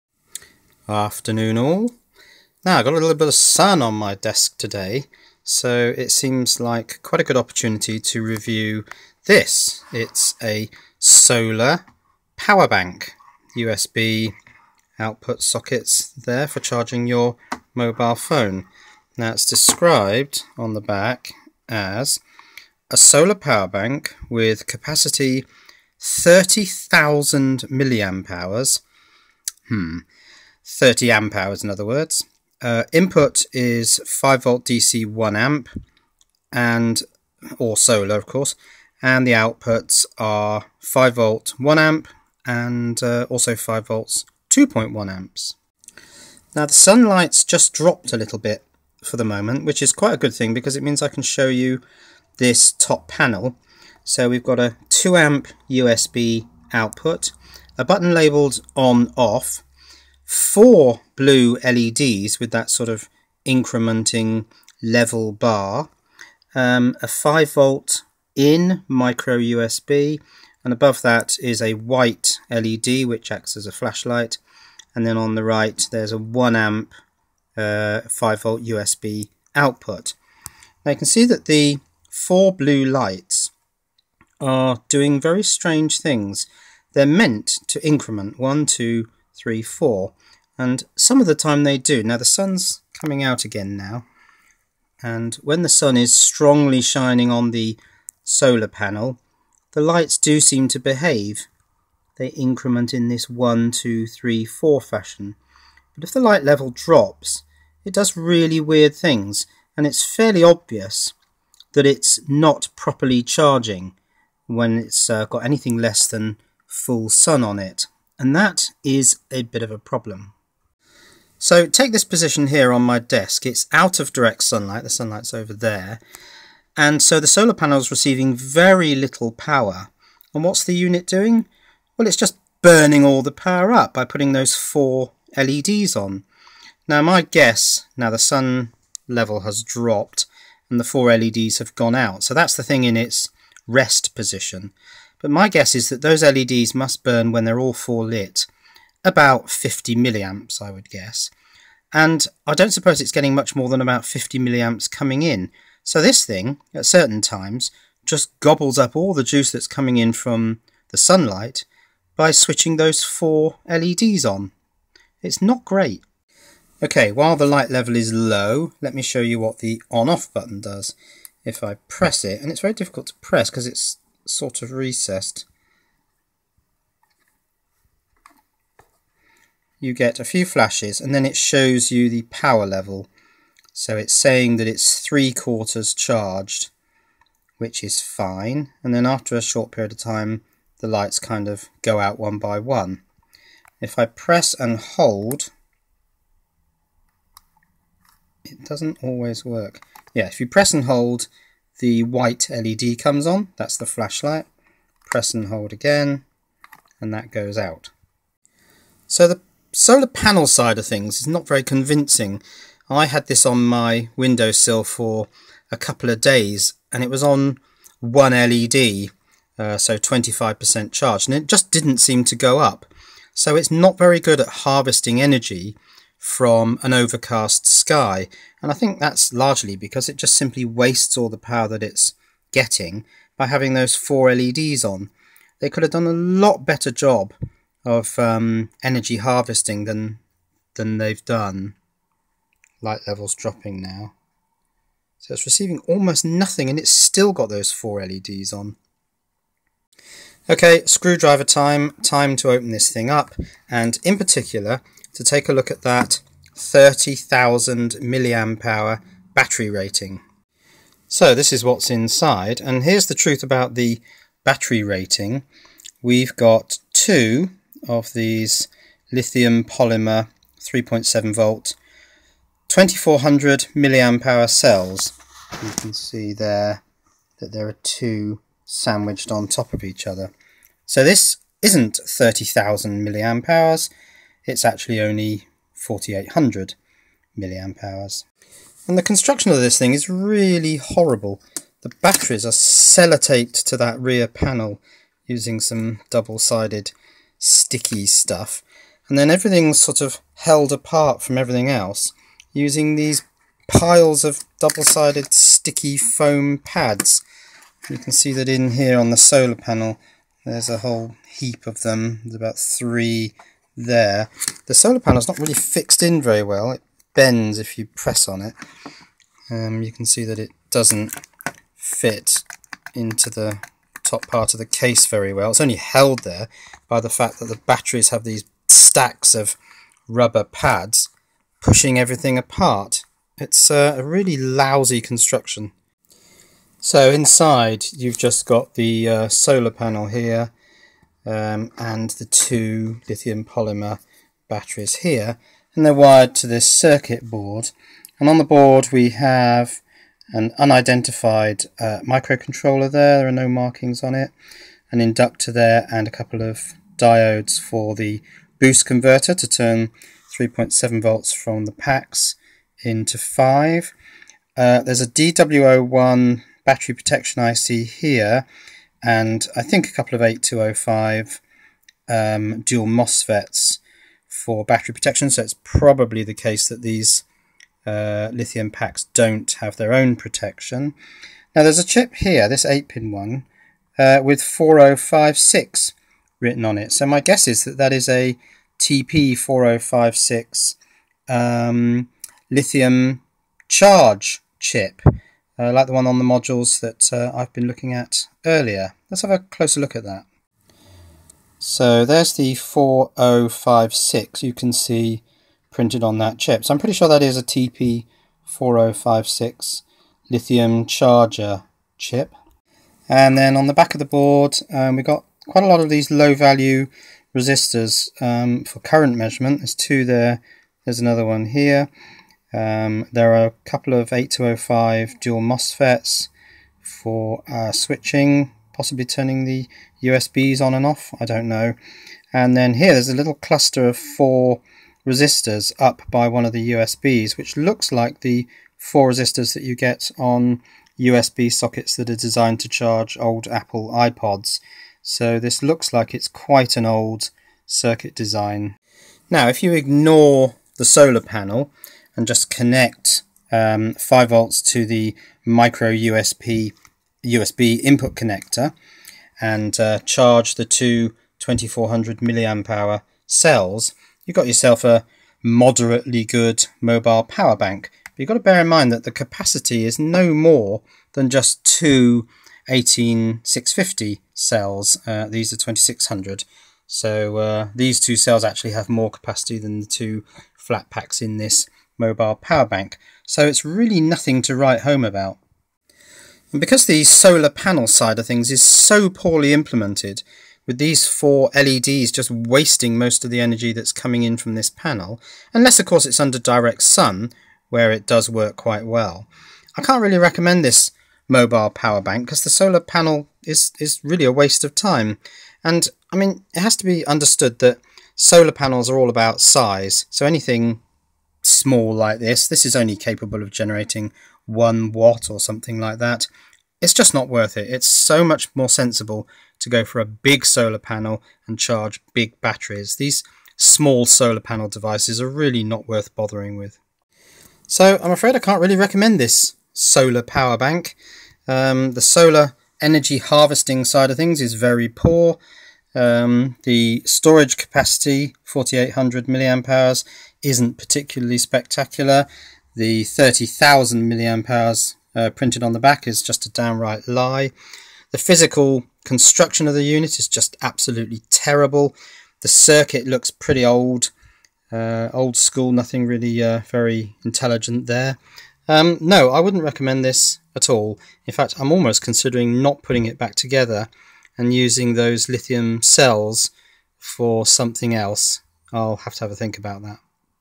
Review: 30,000mAh Solar Power Bank - It's a pile of Junk!
eBay is awash with these solar mobile power banks, most claiming way more storage capacity than is genuinely inside - this one has just 4,800mAh of LiPos. The unit only charges properly in full sun - in low light it wastes all the energy it receives by burning it up in 4 blue LEDs that seem to stay on continuously. The physical construction is dreadful - everything inside is held together with sticky tape and foam pads. Even the circuitry on the PCB is nothing special. It's a waste of money - don't buy it.